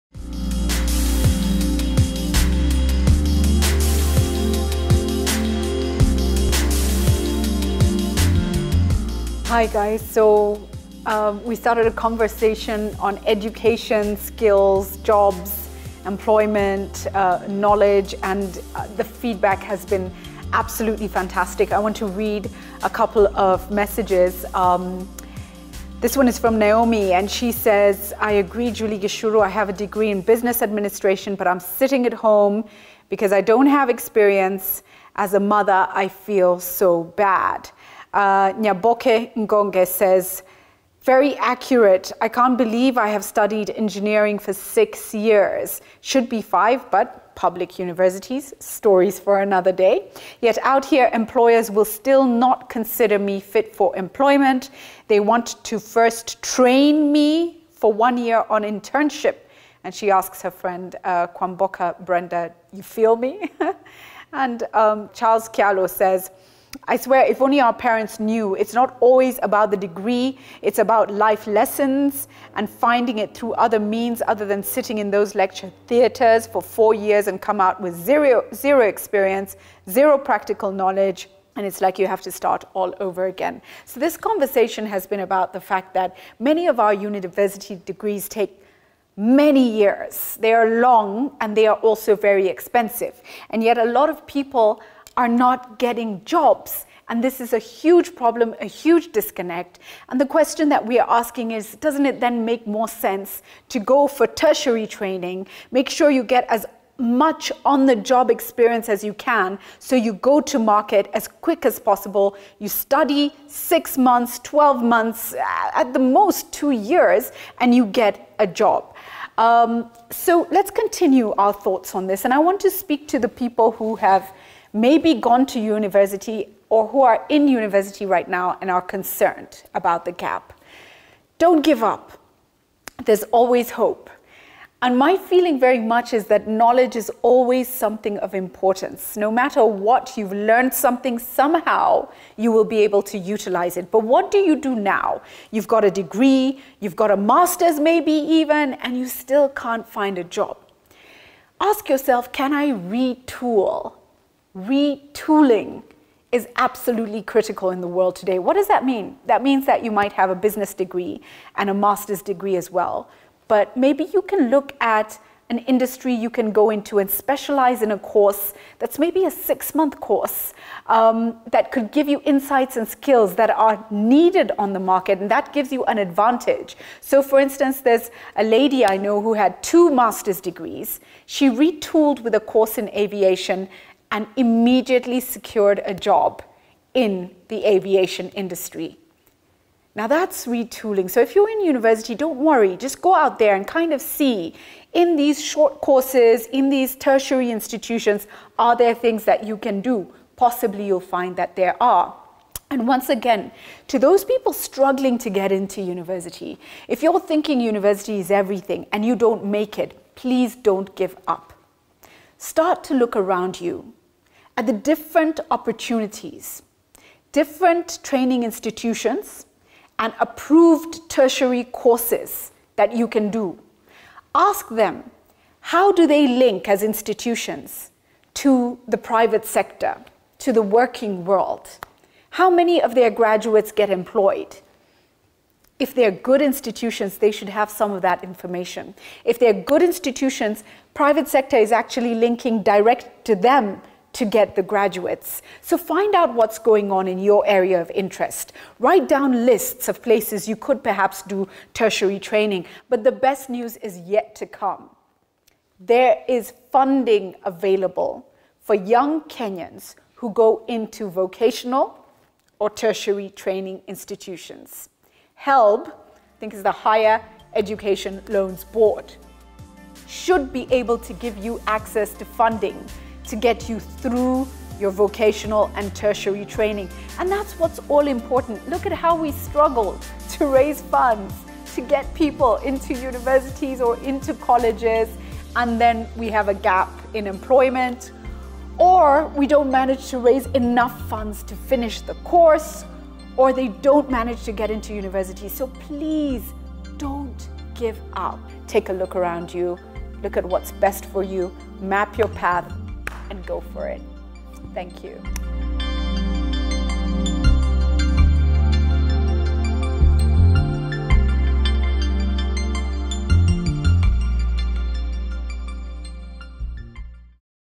Don't Give Up Retool
We are facing a serious skills gap but don't give up. You can plan & retool. Take a look around you, map your path and go for it.